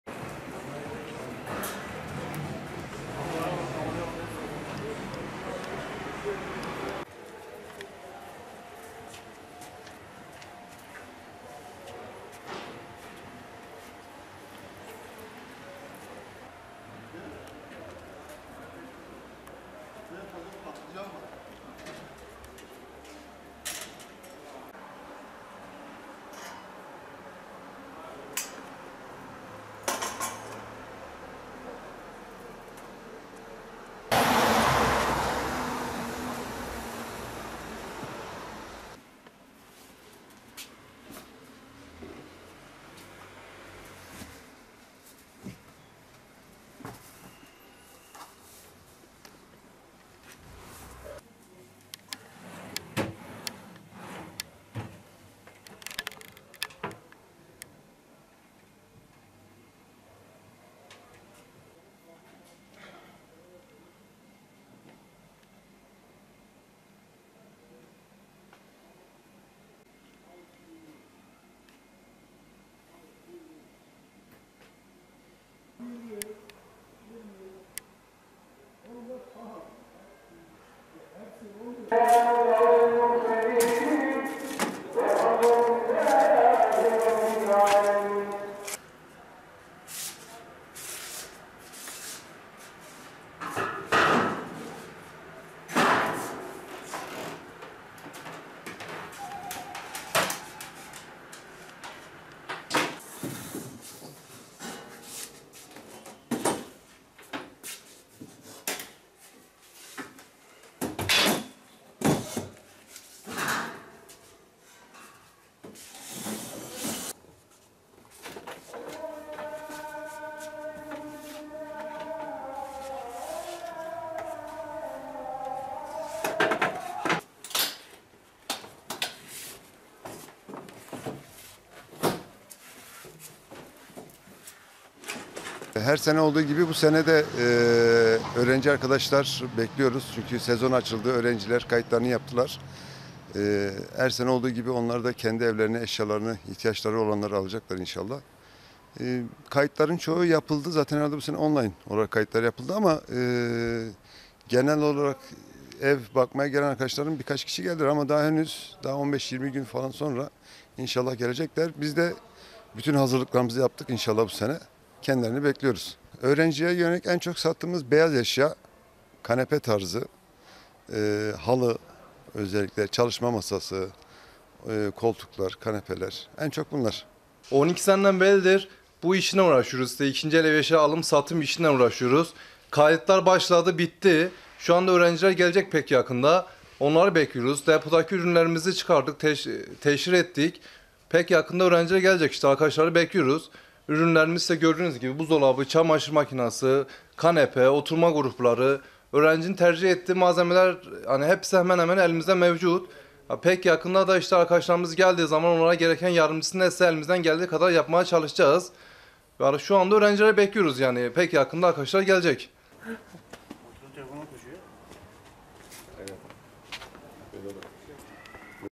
Spot ve ikinci el eşyası satan esnaflar, öğrencileri bekliyor
Elazığ'da 48 bin öğrenciye ulaşan Fırat Üniversitesi'ne 6 bin 769 yeni öğrencinin kayıt yaptırmasıyla birlikte, spot ve ikinci el eşya satan esnaf hazırlıklarını yaparak onları müşteri beklemeye başladı.